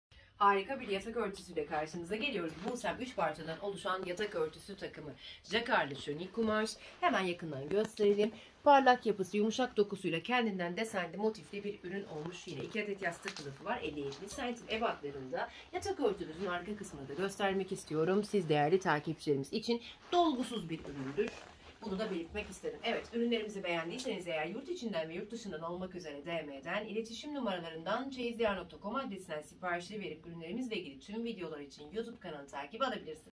Busem Jakarlı Şönil Yatak Örtüsü Gri
Yatak Örtüsü : 235x250 cm
Yastık Kılıfı : 50x70 cm 2 adet

Ürün Kumaşı Jakarlı Şönil Pike Kumaşıdır. Kadifemsi yumuşak bir dokuya sahiptir. Elyaf Dolgusu üründe mevcut değildir.